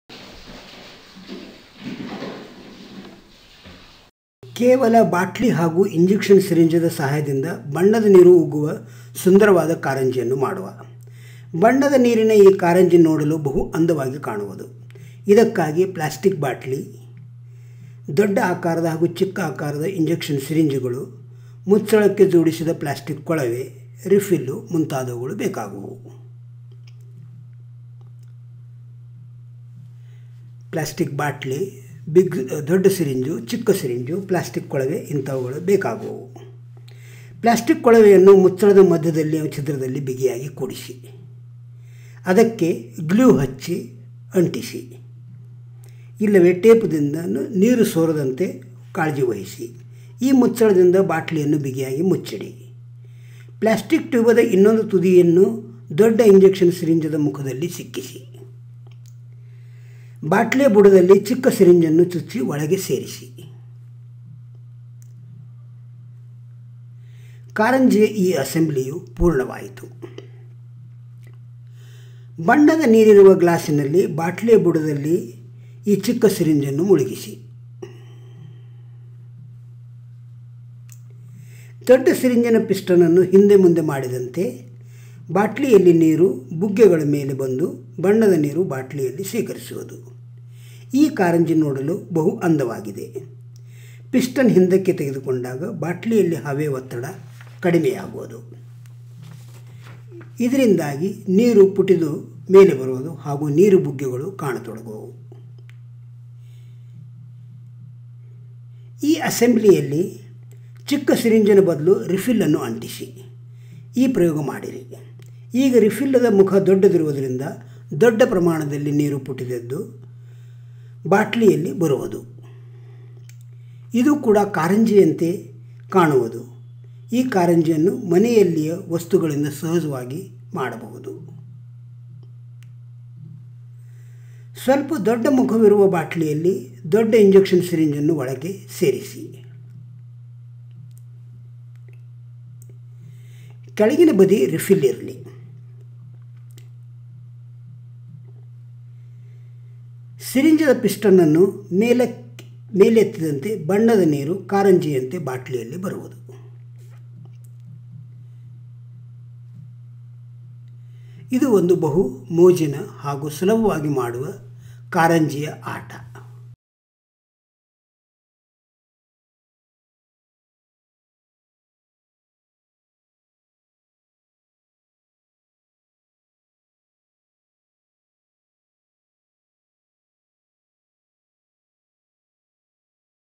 Water Fountains | Kannada
Today we will make an interesting fountain using a bottle and a plastic syringe. As we pull the piston of the syringe water enters the bottle like a fountain from the needle below. To make this fountain you will need a plastic bottle, a 60-ml new syringe, injection needle and a plastic flexi tube. Fix the flexi tube tightly on the bottle lid. There should be no leakages from the joint. Screw the cap tightly on the bottle. Attach a 60-ml syringe on the other end of the flexi tube. Poke a new injection needle in the base of the bottle. Dip the tube attached to the needle in a glass of colored water. Then pull the syringe piston down. This will create a partial vacuum in the bottle and colored water will be sucked through the needle like a fountain. Keep pumping the piston inside-out to continue the fountain.
In this experiment the syringe needle has been replaced with a ball pen refill. This time instead of a thin fountain you will see large quantities of color water gush in through the fountain. We can make this fountain at home using simple
equipment.
Finally take a plastic bottle with a large mouth and tight fit the 60-ml syringe in it. Press fit a ball pen refill in the base of the bottle. Dip the lower end of the refill in water. On pulling the piston up colored water will gush into the bottle like a splendid fountain.